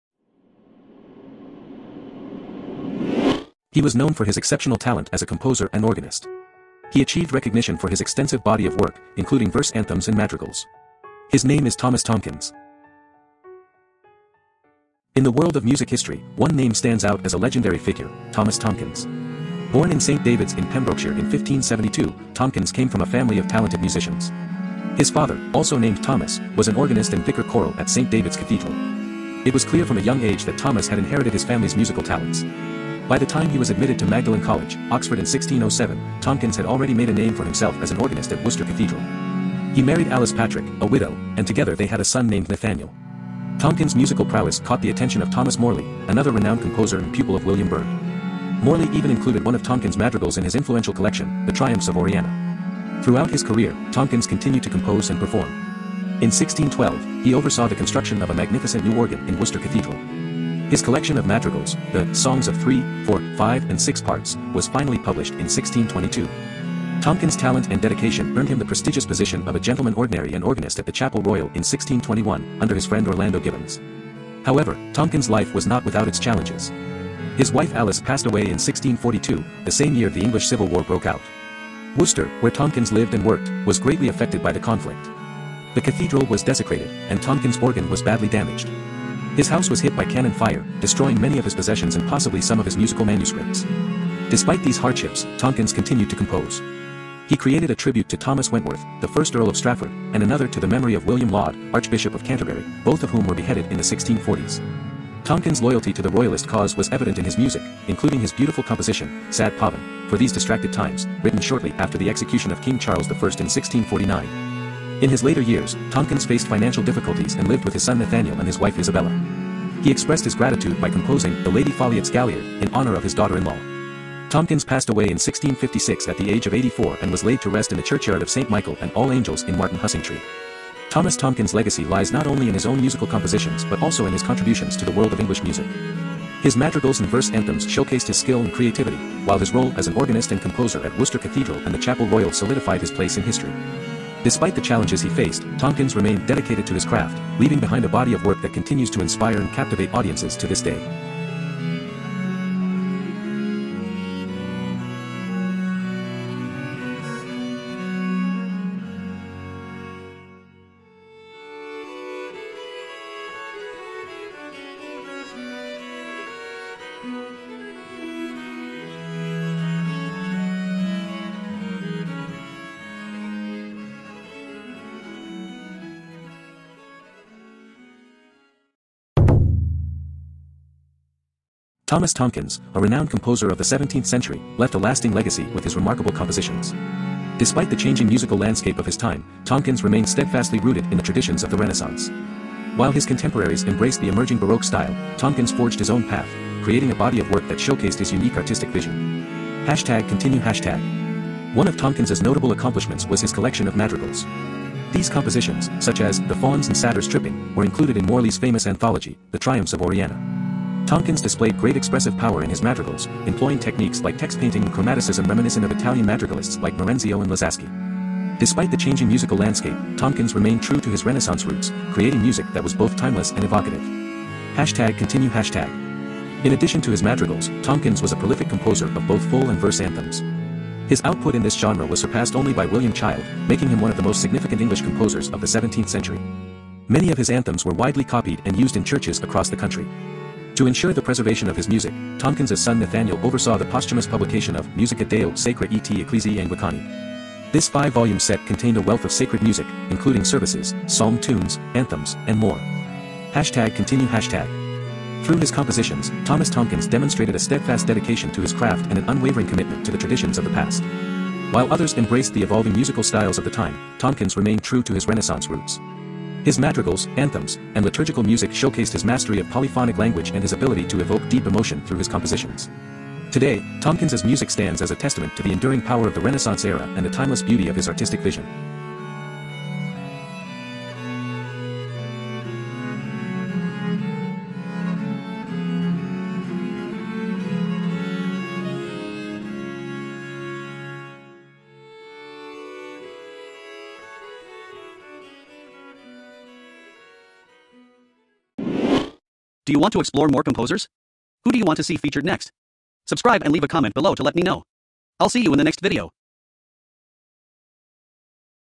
Thomas Tomkins: Musical Mastermind | Composer & Arranger Biography
Tomkins was born in St David's in Pembrokeshire in 1572. His father, also Thomas, who……